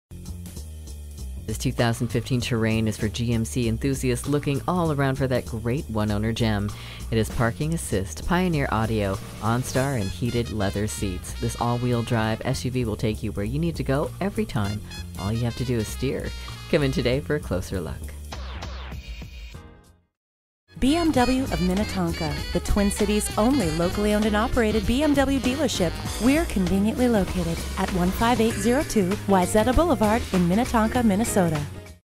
Used 2015 GMC Terrain Minnetonka MN Minneapolis, MN P50616 - SOLD
Year: 2015
Make: GMC
Model: Terrain
Trim: AWD 4dr Denali 3.6L V6
Engine: 3.6 L V6 Cyl.
Transmission: 6-Speed A/T
Color: Quicksilver Metallic
Interior: Jet Black
Mileage: 18510
Stock #: P50616
VIN: 2GKFLZE30F6216346
AWD. Silver Bullet! Move quickly! This 2015 Terrain is for GMC fanatics looking all around for a great one-owner gem. This SUV will take you where you need to go every time...all you have to do is steer!

Address:
15802 Wayzata Blvd